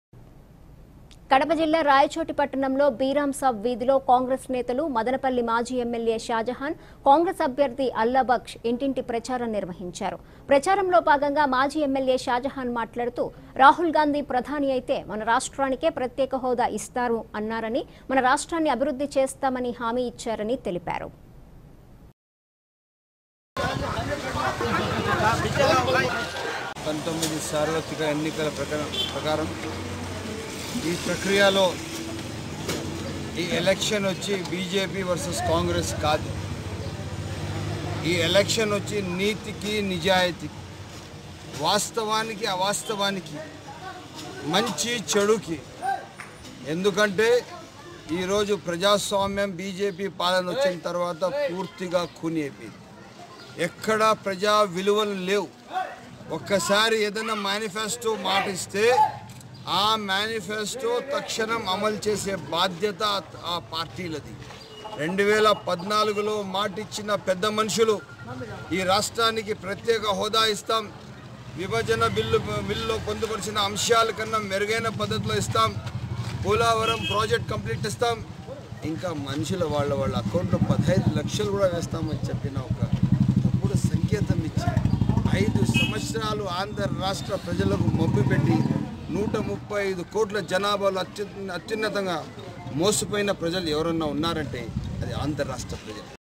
కడప జిల్లా రాయచోటిలో కాంగ్రెస్ నేతలు మాజీ ఎమ్మెల్యే షాజహాన్ ,కాంగ్రెస్ అభ్యర్ధి అల్లాభక్షు ప్రచారం
కడప జిల్లా రాయచోటి పట్టణంలో బీరాంసాబ్ వీధిలో కాంగ్రెస్ నేతలు మదనపల్లి మాజీ ఎమ్మెల్యే షాజహాన్ ,కాంగ్రెస్ అభ్యర్ధి అల్లాభక్షు ఇంటింటి ప్రచారం నిర్వహించారు. ప్రచారంలో భాగంగా మాజీ ఎమ్మెల్యే షాజహాన్ మాట్లాడుతూ రాహుల్ గాంధీ ప్రధాని అయితే మన రాష్టానికే ప్రత్యేక హోదా ఇస్తారన్నారని,  మన రాష్టాన్ని అభివృద్ధి చేస్తామని హామీ ఇచ్చారని తెలిపారు.